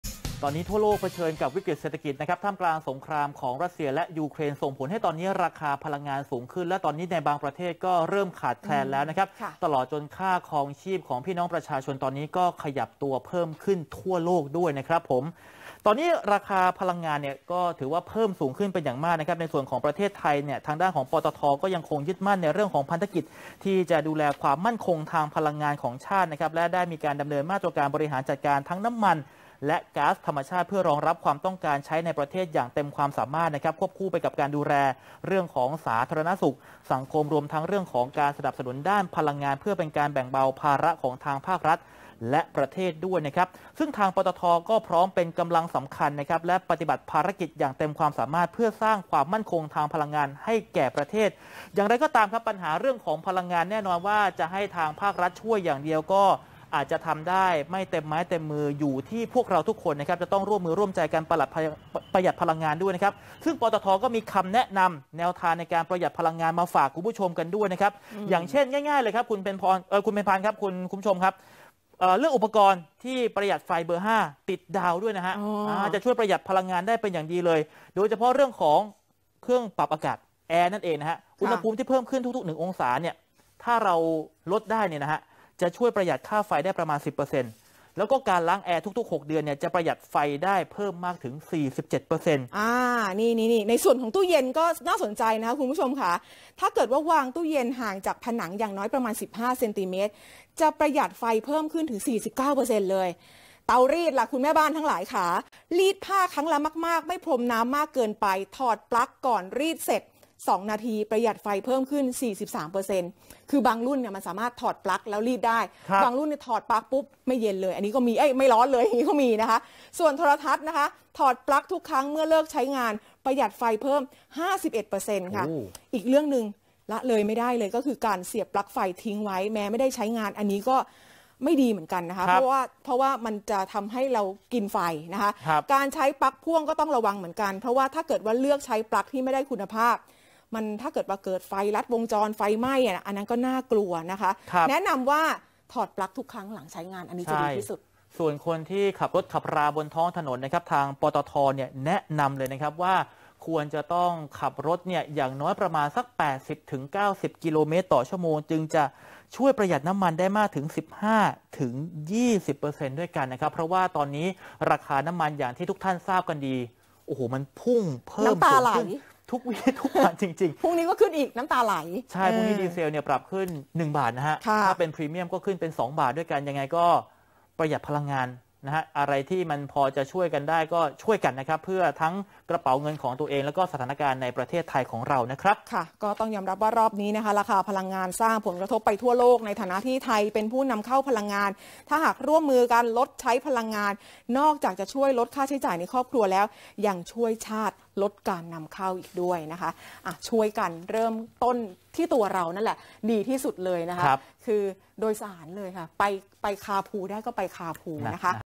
ปตท.ชวนคนไทยร่วมประหยัดพลังงานฝ่าวิกฤติราคาพุ่ง
ตอนนี้ทั่วโลกเผชิญวิกฤติเศรษฐกิจ ท่ามกลางสงครามรัสเซีย-ยูเครน ส่งผลให้ราคาพลังงานสูงขึ้น และเริ่มขาดแคลนในบางประเทศ ตลอดจนค่าครองชีพของประชาชนที่ปรับตัวสูงขึ้นทั่วโลก

ในประเทศไทย ปตท. ยังคงยึดมั่นพันธกิจดูแลความมั่นคงทางพลังงานของชาติ และได้ดำเนินมาตรการบริหารจัดการน้ำมันและก๊าซธรรมชาติ รองรับความต้องการใช้ในประเทศอย่างเต็มความสามารถ ควบคู่ไปกับการร่วมดูแลสาธารณสุขและสังคม รวมถึงสนับสนุนด้านพลังงาน เพื่อแบ่งเบาภาระของภาครัฐและประเทศ เช่น ขยายระยะเวลาช่วยเหลือส่วนลดค่าซื้อก๊าซ LPG แก่ผู้มีรายได้น้อย กลุ่มร้านค้า หาบเร่ แผงลอยอาหาร ที่มีบัตรสวัสดิการแห่งรัฐ จำนวนเงิน 100 บาท/คน/เดือน จนถึง 30 มิถุนายนนี้

ปตท.พร้อมเป็นกำลังสำคัญ และปฏิบัติภารกิจอย่างเต็มความสามารถ เพื่อสร้างความมั่นคงทางพลังงานให้แก่ประเทศ อย่างไรก็ตาม เสถียรภาพทางพลังงานของประเทศจะเกิดขึ้นอย่างแท้จริง อยู่ที่ทุกคนร่วมมือร่วมใจประหยัดและรู้ใช้พลังงานอย่างมีประสิทธิภาพด้วย ปตท.ก็มีคำแนะนำแนวทางประหยัดพลังงาน เช่น เลือกใช้อุปกรณ์ไฟฟ้าที่มีฉลากประหยัดไฟฟ้าเบอร์ 5 ติดดาว

เครื่องปรับอากาศ : อุณหภูมิที่เพิ่มขึ้น 1 องศาฯ จะช่วยลดค่าไฟฟ้าประมาณ 10% และการล้างแอร์ทุก 6 เดือน ประหยัดไฟเพิ่มขึ้น 47%
 ตู้เย็น : วางตู้เย็นห่างจากผนังอย่างน้อย 15 เซนติเมตร ประหยัดไฟเพิ่มขึ้น 49%
เตารีด : รีดผ้าครั้งละมากๆ ไม่พรมน้ำมากเกินไป และถอดปลั๊กก่อนรีดเสร็จ 2 นาที ประหยัดไฟเพิ่มขึ้น 43%
โทรทัศน์ : ถอดปลั๊กทุกครั้งเมื่อเลิกใช้งาน ประหยัดไฟเพิ่มขึ้น 51%

อีกหนึ่งเรื่องที่อาจจะละเลยไป คือ การเสียบปลั๊กไฟทิ้งไว้ แม้ไม่ได้ใช้งาน - การใช้ปลั๊กพ่วง ก็ต้องระวังเช่นกัน เพราะหากเลือกใช้ปลั๊กไม่ได้คุณภาพ อาจทำให้ไฟลัดวงจรและเกิดไฟไหม้ได้ ดังนั้น ถ้าไม่ได้ใช้งาน แนะนำให้ถอดปลั๊กทุกครั้ง

ส่วนการบรรทุกบนรถกระบะ ปตท.แนะนำให้จัดวางของกึ่งกลางให้น้ำหนักสมดุลกัน ไม่ควรวางให้น้ำหนักลงด้านใดด้านหนึ่ง - ขับรถด้วยความเร็วคงที่ วิ่งทางไกลควรชิดซ้าย

และรถทั้งกลุ่มเบนซิน กลุ่มดีเซล ปตท.แนะนำว่าควรใช้ความเร็วสม่ำเสมอ 80-90 กิโลเมตร/ชั่วโมง ช่วยประหยัดน้ำมันได้มากถึง 15-20% ถ้าลดการบรรทุกสิ่งของที่ไม่จำเป็นออก ก็จะช่วยประหยัดน้ำมันได้ด้วย ความจริงแล้วขับช้าเกินไปก็กินน้ำมัน ขับเร็วเกินไปก็ไม่ประหยัด ทางที่ดีที่สุดคือขับความเร็วคงที่ 90 กิโลเมตร/ชั่วโมง หรือตามที่บริษัทรถยนต์แนะนำ ก็ช่วยประหยัดน้ำมันได้แล้วครับ

รอบนี้ราคาพลังงานสร้างผลกระทบไปทั่วโลก ในฐานะไทยเป็นผู้นำเข้าพลังงาน หากร่วมมือกันลดใช้พลังงาน นอกจากจะช่วยลดค่าใช้จ่ายในครอบครัวแล้ว ยังช่วยชาติลดการนำเข้าอีกด้วย